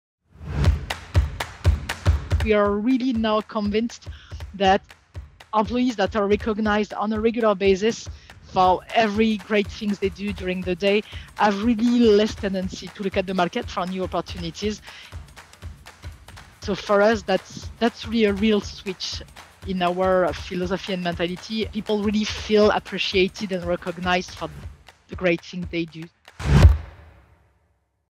Achievers supports retention at Criteo
Hear from Criteo's Total Reward Director, Lorine Piasentin, on how regular recognition through their Achievers program is reducing employees' desire to job hunt.